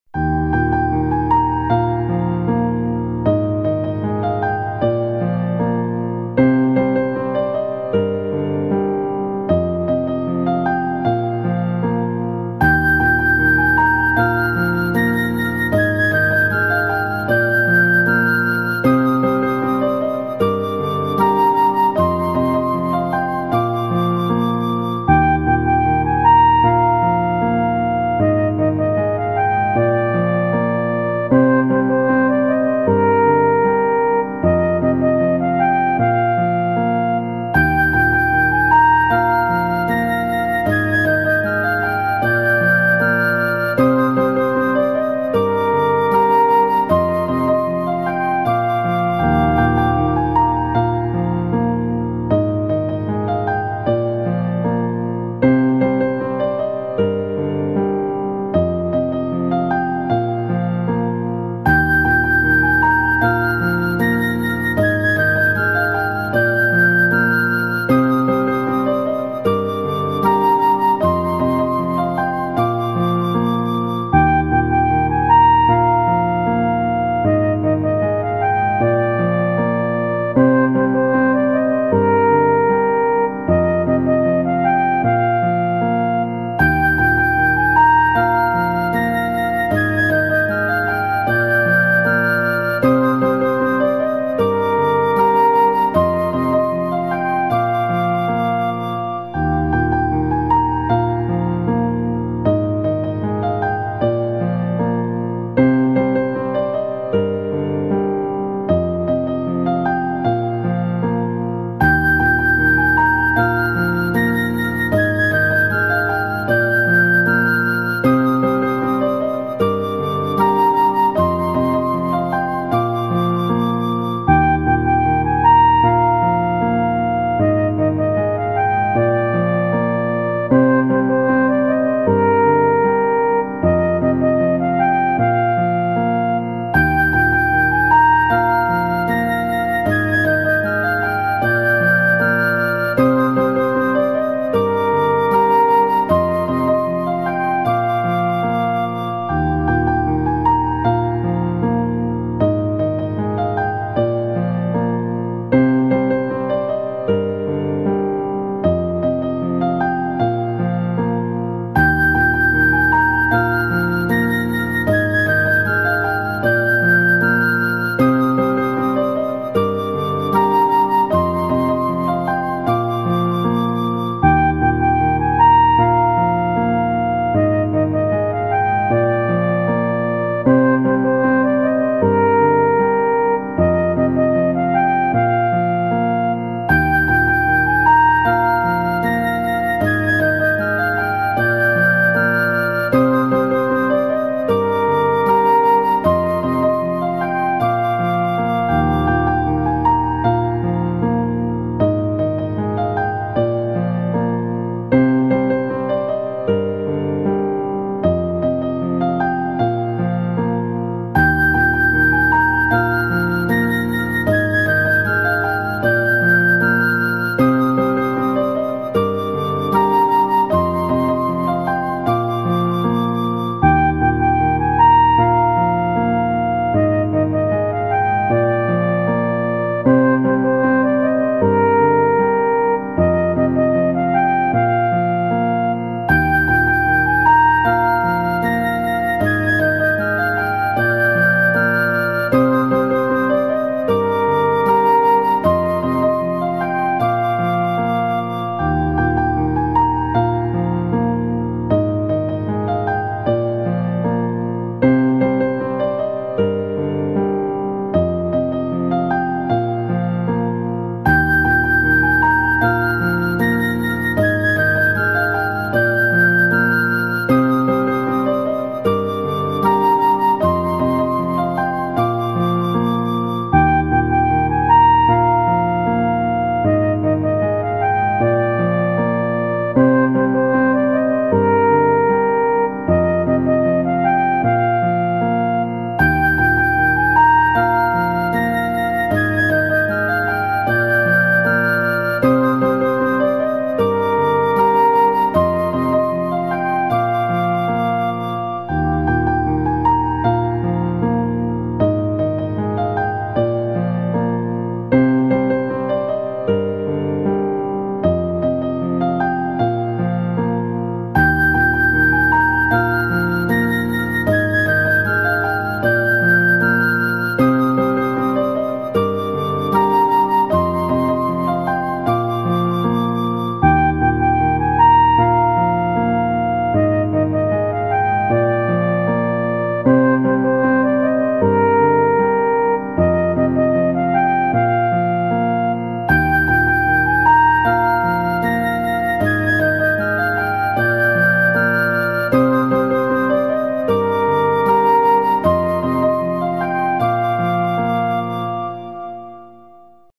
马来西亚怡保～槟城 第4天《初到槟城》2023-01-03
今天准备从马来西亚怡保火车站乘坐火车去槟城，在那里已订了2天私人公寓式酒店，隨着火车的启动，跟我去槟城溜达溜达吧！